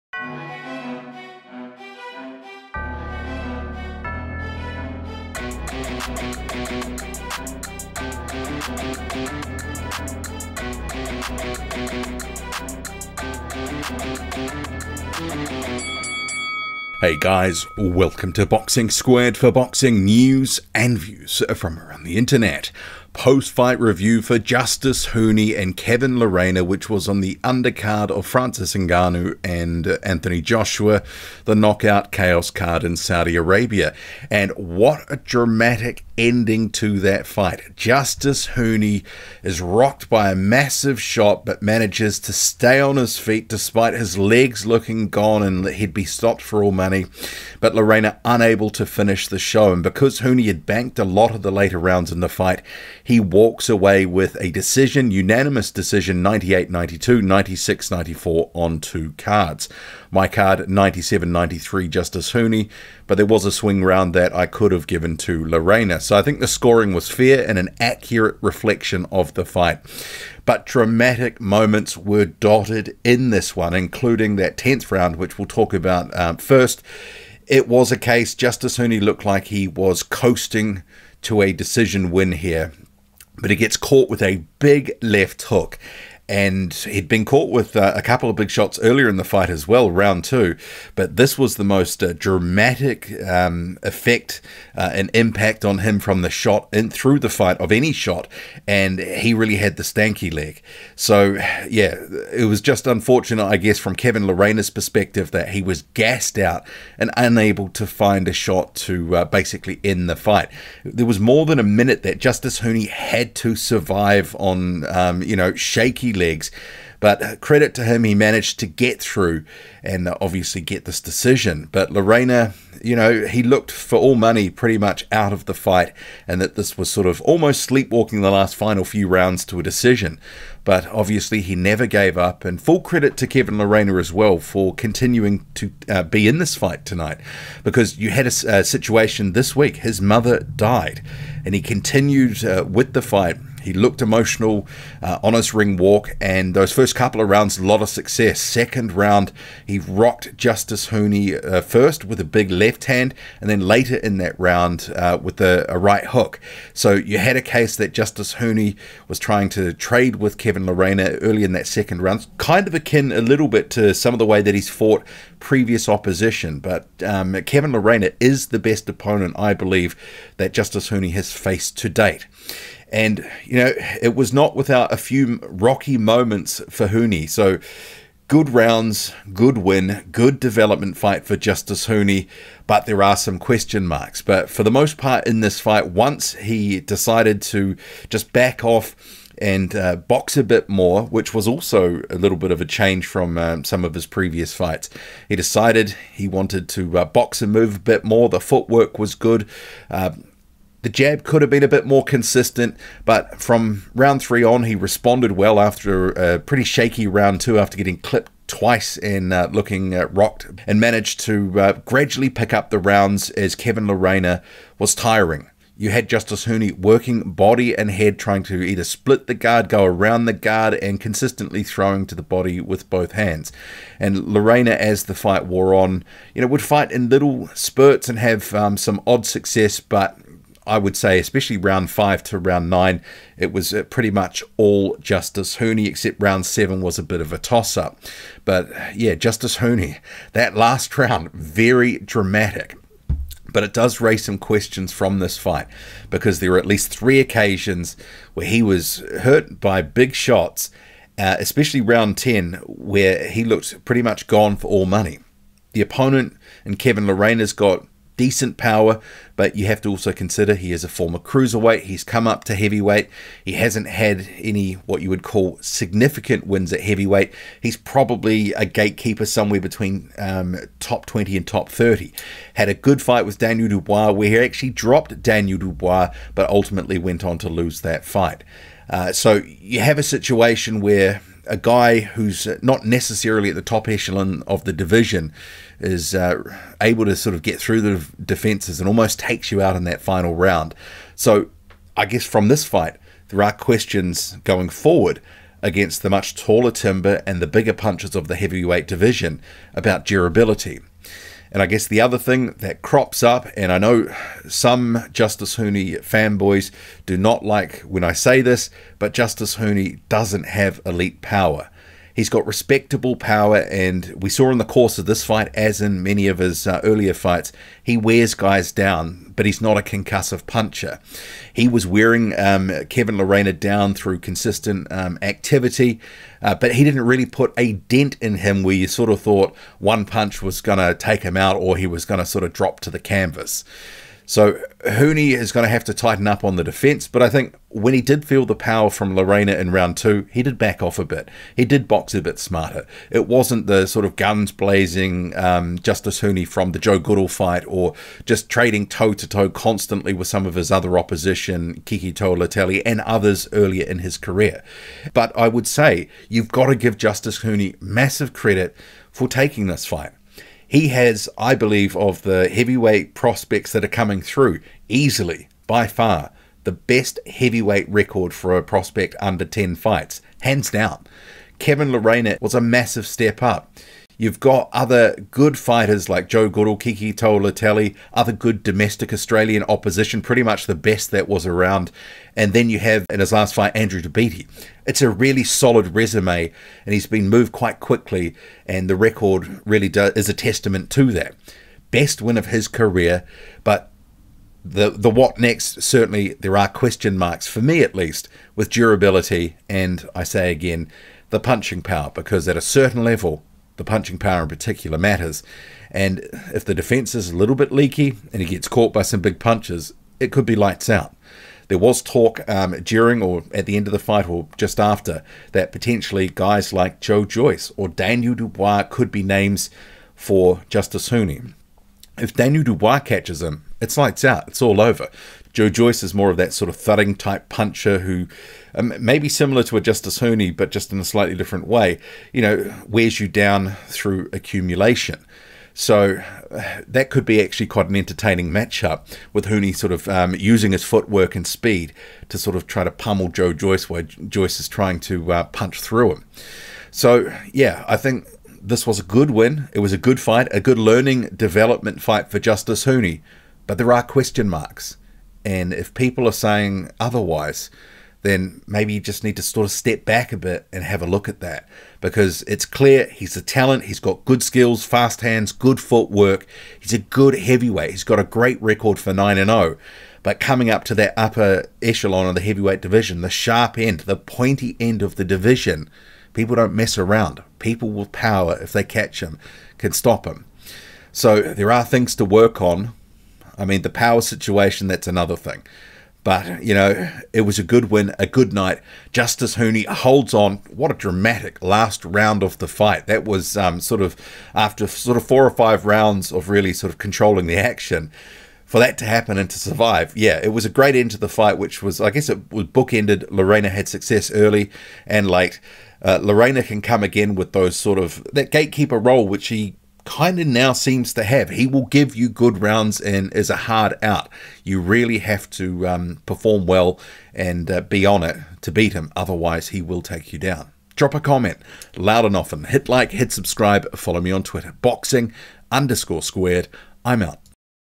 JUSTIS HUNI SURVIVES DRAMATIC LAST ROUND TO DECISION KEVIN LERENA
Justis Huni saw off some rocky moments in round 2 and a dramatic final round where he was badly hurt, to grab a unanimous decision over Kevin Lerena (98-92, 96-94, 96-94) in Saudi Arabia on the Joshua/Ngannou undercard. The Australian moved to 9-0.

Boxing Squared

World Heavyweight Boxing